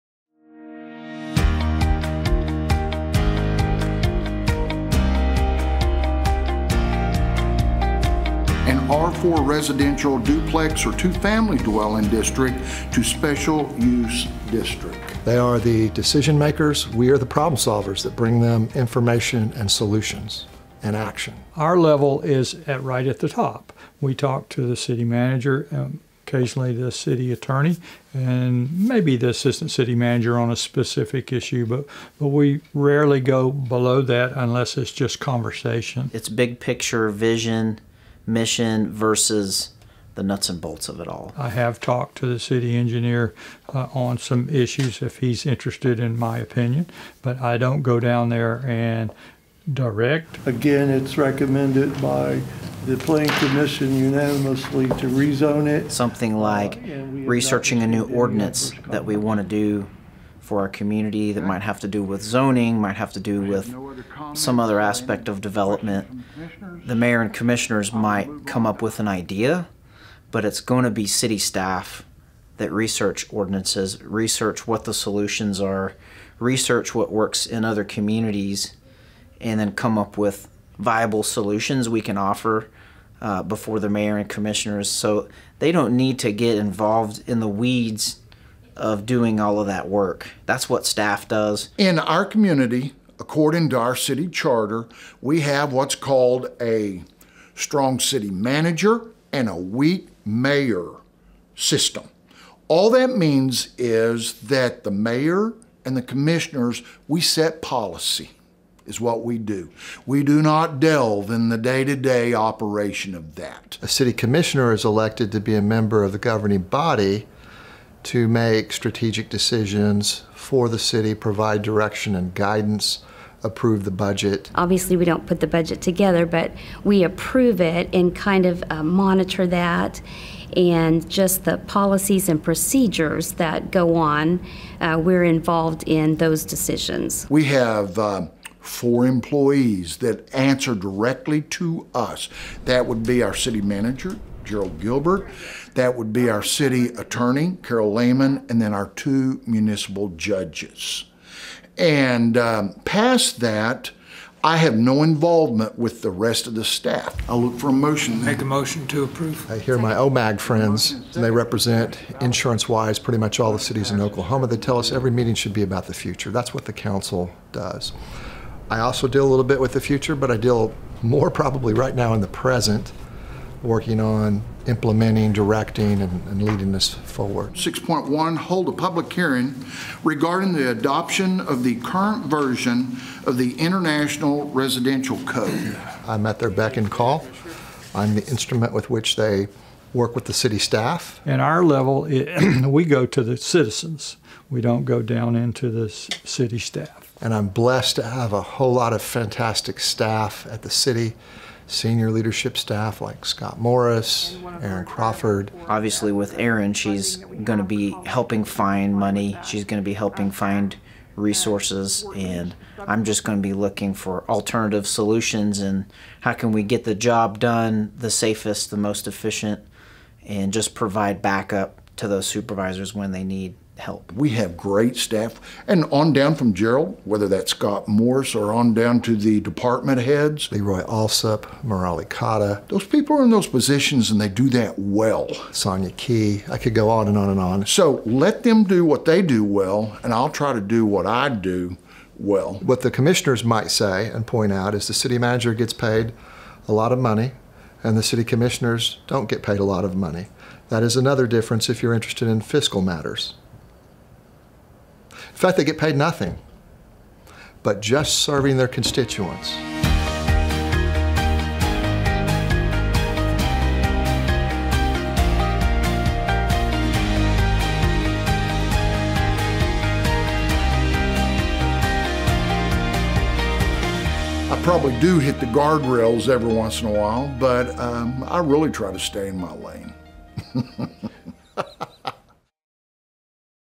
Duties of City Management vs. City Commission
Members of the City Commission and City Management talk about their different roles. Mayor David Mason, Ward 1 Commissioner Cheryl Patterson, and Ward 5 Commissioner Rob Stallings speak from the perspective of elected officials. City Manager Jerald Gilbert and Assistant City Manager Scott Morris speak from the hired management perspective.